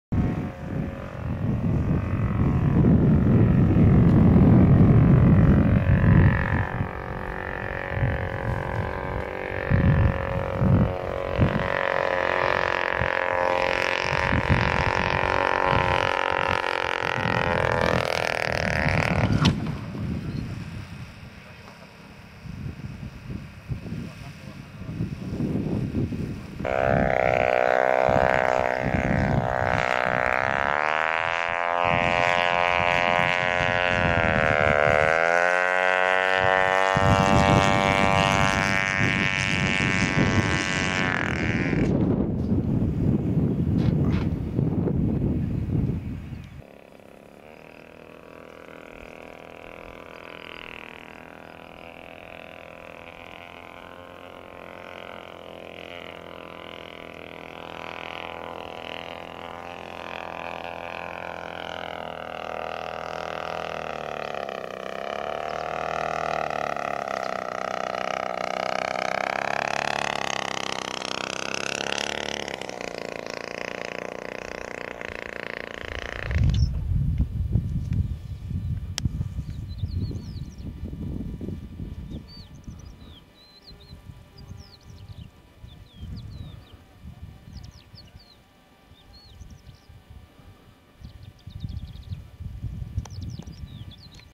UAV Fulmar Aerovision 2010 Net Landings
Aerovision Fulmar landing on the net